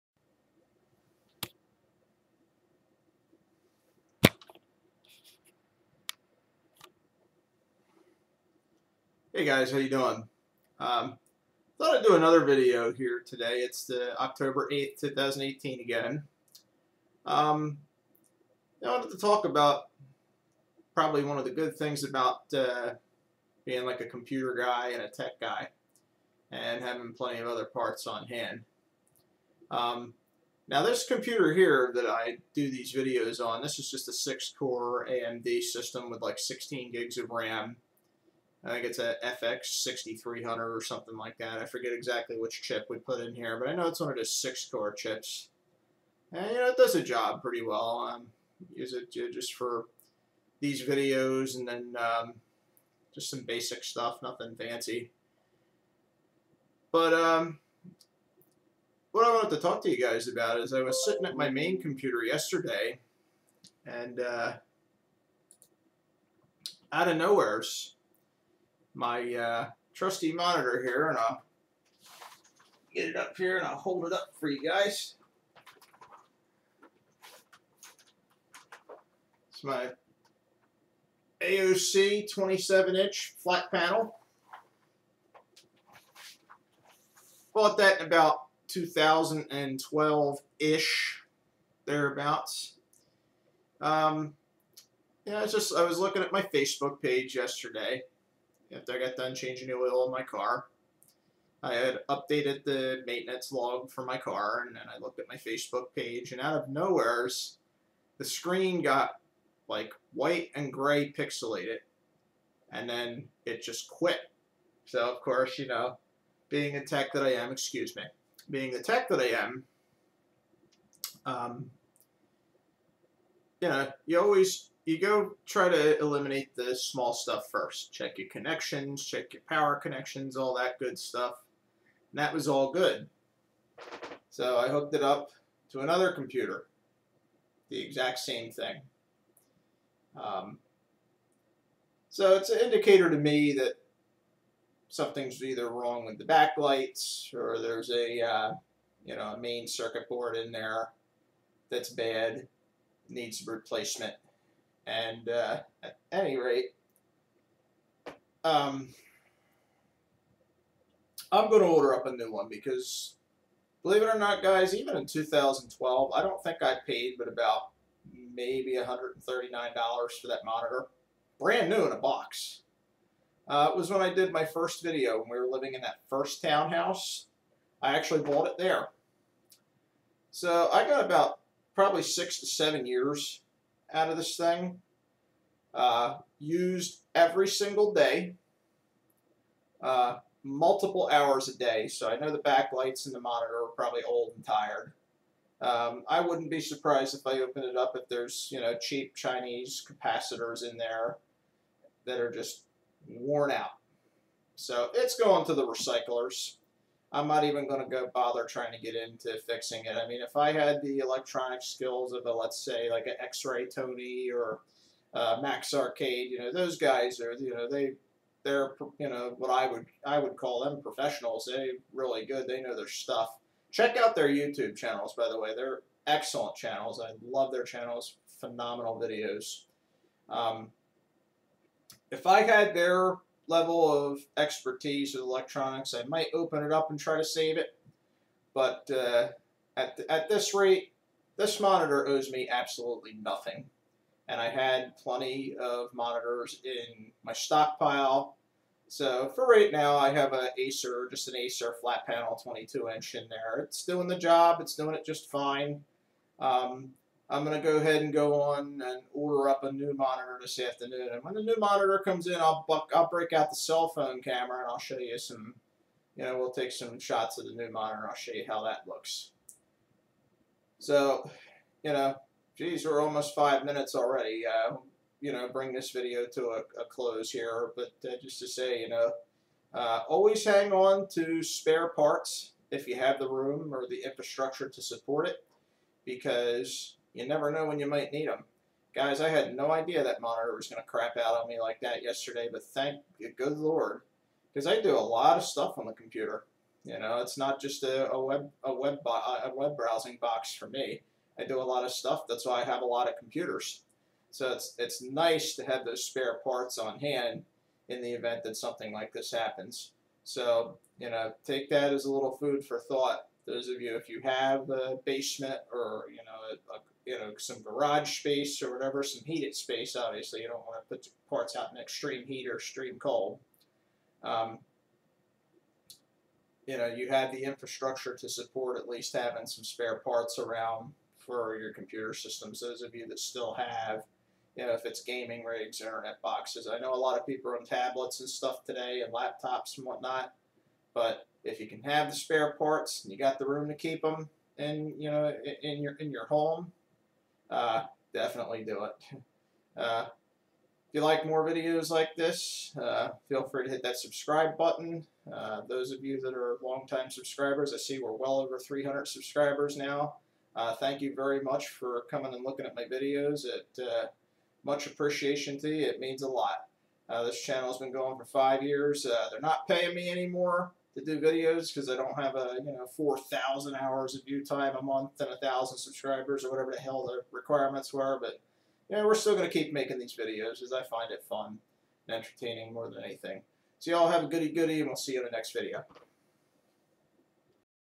My AOC 27" Flat screen Monitor Finally bit the dust.
6+ years of solid everyday use for multiple hours a day. It's off to the recycle center  now, and I'm ordering up a new one. Will do a video when the new one gets here.